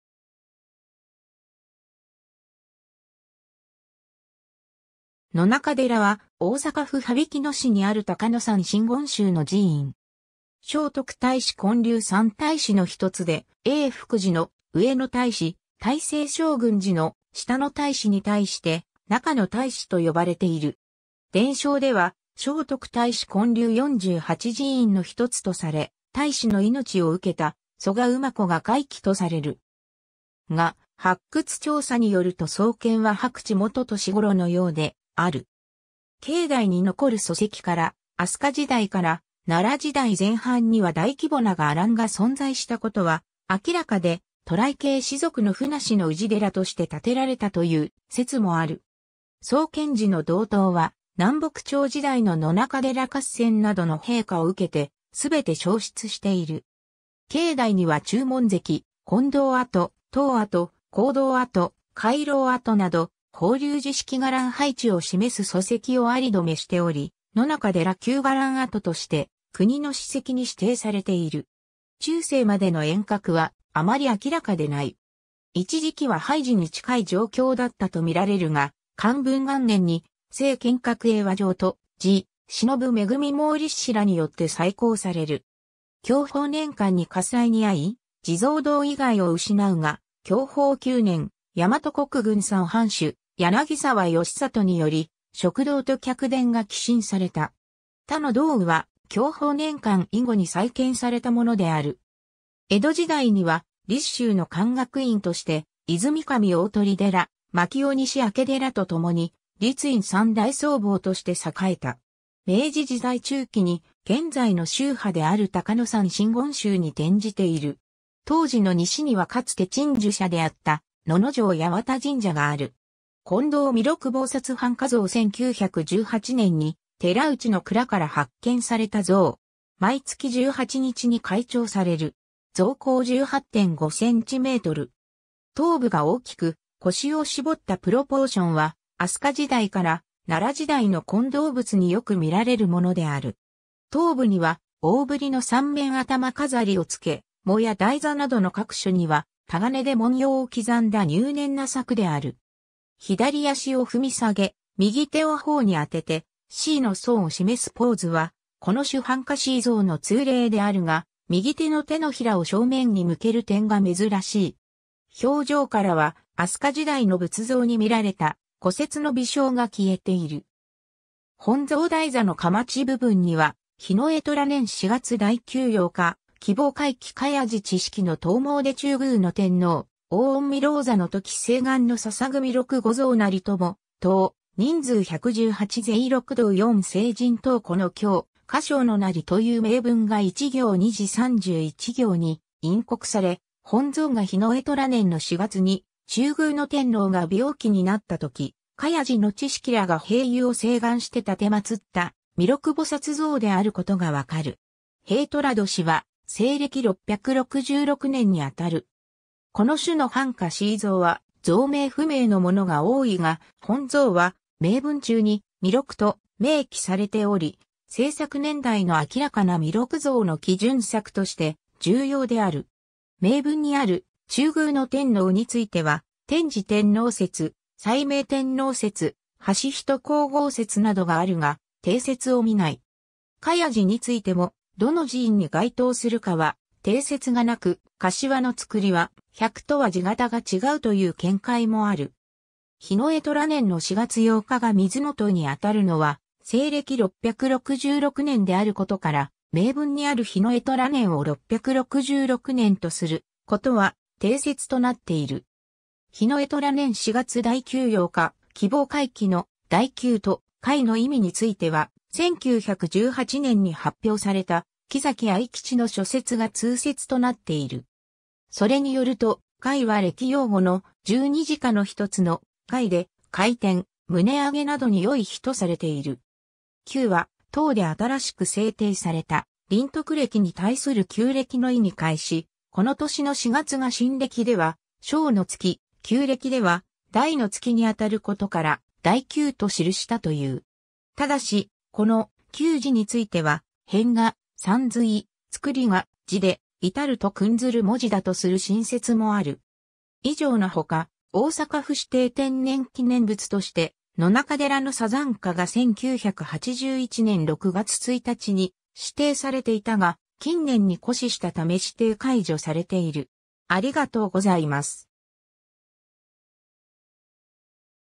野中寺
野中寺（やちゅうじ）は、大阪府羽曳野市にある高野山真言宗の寺院。
聖徳太子建立三太子の一つで、叡福寺（太子町）の「上の太子」、大聖勝軍寺（八尾市）の「下の太子」に対して、「中の太子」と呼ばれている。
 伝承では聖徳太子建立48寺院の一つとされ、太子の命を受けた蘇我馬子が開基とされる。
が、発掘調査によると創建は白雉元年（650年）頃のようである。
境内に残る礎石から、飛鳥時代から奈良時代前半には大規模な伽藍が存在したことは明らかで、渡来系氏族の船氏の氏寺として建てられたという説もある。
 創建時の堂塔は南北朝時代の野中寺合戦などの兵火を受けて全て焼失している。
境内には中門跡・金堂跡・塔跡・講堂跡・回廊跡など法隆寺式伽藍配置を示す礎石を存留しており、「野中寺旧伽藍跡」として国の史跡に指定されている。
 中世までの沿革はあまり明らかでない。
一時期は廃寺に近い状況だったと見られるが、寛文元年（1661年）に政賢覚英和上と慈忍恵猛律師らによって再興される。
 享保年間（1716年 - 1735年）に火災にあい、地蔵堂以外を失うが、享保9年（1724年）、大和国郡山藩主柳沢吉里により食堂と客殿が寄進された。
他の堂宇は享保年間以後に再建されたものである。
 江戸時代には律宗の勧学院として、和泉神鳳寺（大鳥大社境内にあった）、槇尾西明寺とともに律院三大僧坊として栄えた。
 明治時代中期に現在の宗派である高野山真言宗に転じている。
 当寺の西にはかつて鎮守社であった野々上八幡神社がある。
 金銅弥勒菩薩半跏像 （重要文化財） 1918年（大正7年）に寺内の蔵から発見された像。
毎月18日に開帳される。
像高18.5cm。
頭部が大きく、腰を絞ったプロポーションは、飛鳥時代から奈良時代の金銅仏によく見られるものである。
頭部には大ぶりの三面頭飾を付け、裳や台座などの各所にはタガネで文様を刻んだ入念な作である。
左脚を踏み下げ、右手を頬に当てて思惟の想を示すポーズはこの種半跏思惟像の通例であるが、右手の掌を正面に向ける点が珍しい。
表情からは飛鳥時代の仏像にみられた「古拙の微笑」が消えている。
 本像台座の框（かまち）部分には「丙寅年四月大旧八日癸卯開記　栢寺智識之等詣中宮天皇大御身労坐之時　請願之奉弥勒御像也　友等人数一百十八　是依六道四生人等此教可相之也」（以上の読みには異説もある）という銘文が1行2字・31行（62字）に陰刻され、本像が丙寅年の四月に「中宮天皇」が病気になったとき「栢寺」の知識（信徒）らが平癒を請願して奉った弥勒菩薩像であることが分かる。
丙寅年は西暦666年に当たる。
この種の半跏思惟像は像名不明のものが多いが、本像は銘文中に「弥勒」と明記されており、制作年代の明らかな弥勒像の基準作として重要である。
銘文にある「中宮天皇」については「天智天皇説」「斉明天皇説」「間人皇后説」などがあるが、定説をみない。
「栢寺」についてもどの寺院に該当するかは定説がなく、「栢」の旁は「百」とは字形が違うという見解もある。
 丙寅年の4月8日が癸卯にあたるのは西暦666年（天智天皇5年）であることから、銘文にある丙寅年を666年とすることは定説となっている。
「丙寅年四月大旧八日癸卯開記」の「大旧」と「開」の意味については、1918年（本像発見の年）に発表された木崎愛吉の所説が通説となっている。
それによると、「開」は暦用語の「十二直」の1つの「開」で、開店、棟上げなどに良い日とされている。
「旧」は唐で新しく制定された麟徳暦（儀鳳暦）に対する旧暦（元嘉暦）の意に解し、この年の4月が新暦（麟徳暦）では「小の月」、旧暦（元嘉暦）では「大の月」にあたることから「大旧」と記したという。
ただし、この「旧」字については偏が「さんずい」、旁が「自」で「いたる」と訓ずる文字だとする新説もある。
 以上のほか、大阪府指定天然記念物として「野中寺のさざんか」が1981年（昭和56年）6月1日に指定されていたが、近年に枯死したため指定解除されている。